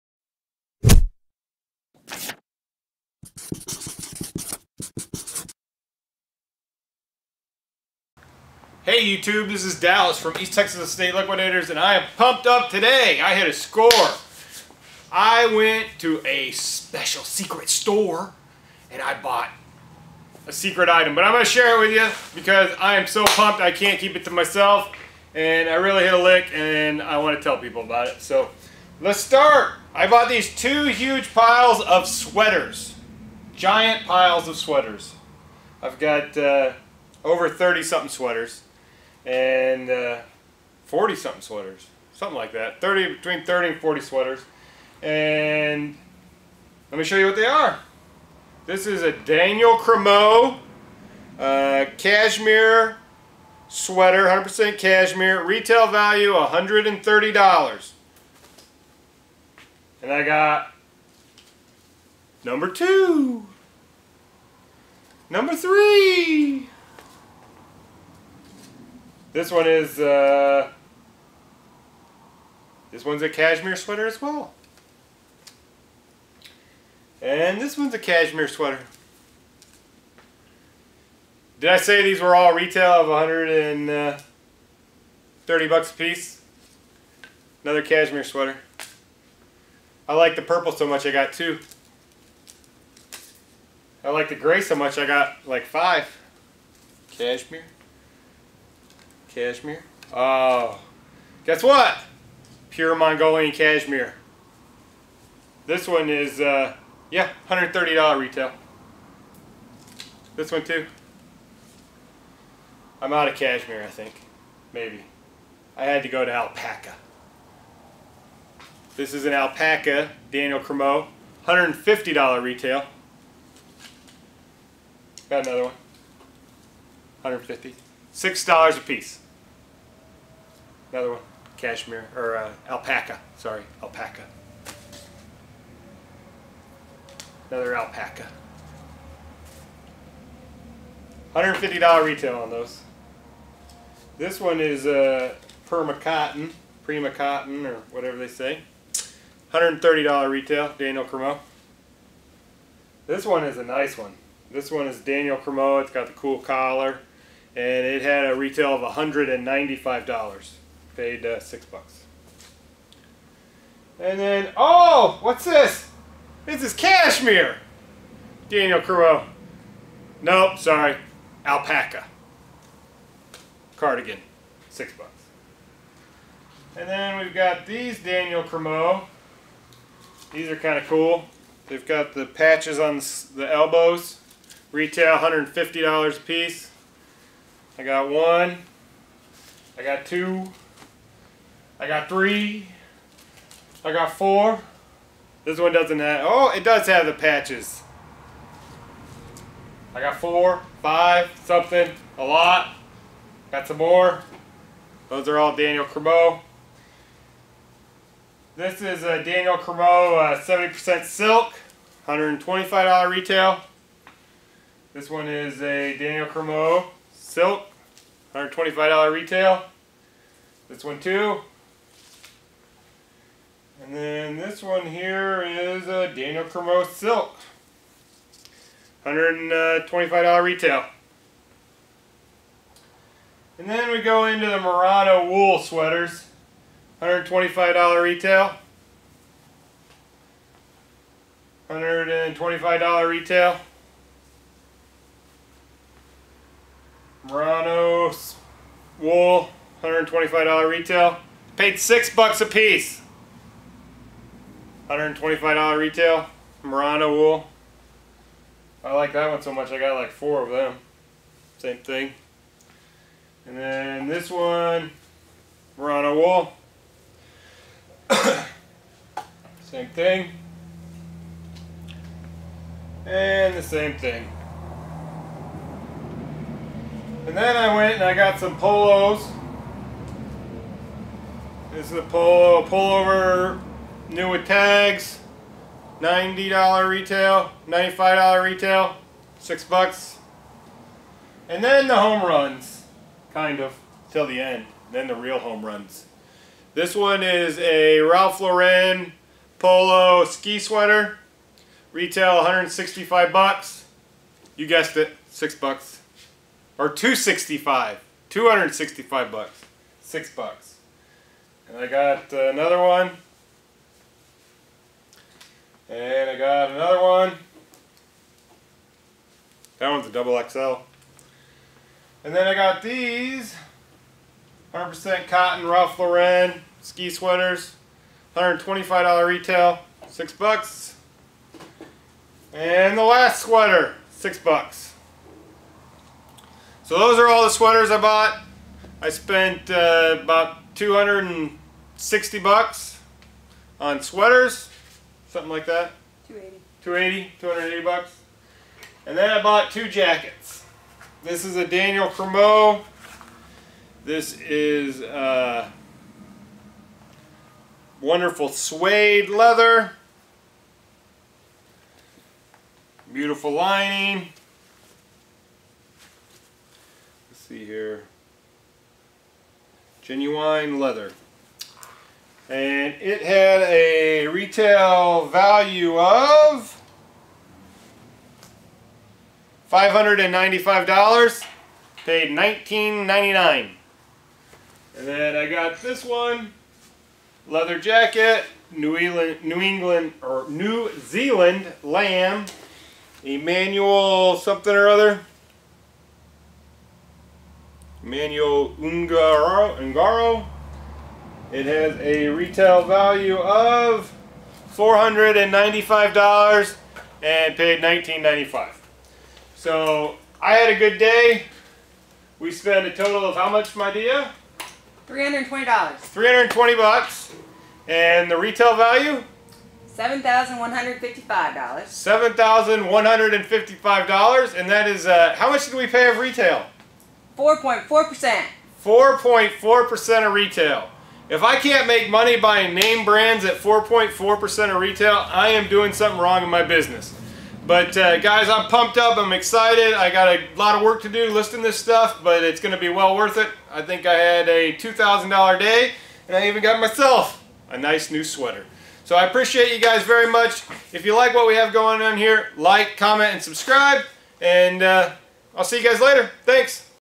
Holy Sweater Bat Man!! $7155 Retail Value for Less Than 5%. Retail Arbitrage
I was in the right place at the right time.  But if I was not looking I would of never found it.  I will turn a less than $400.00 into $2500.00 quick and you can too!!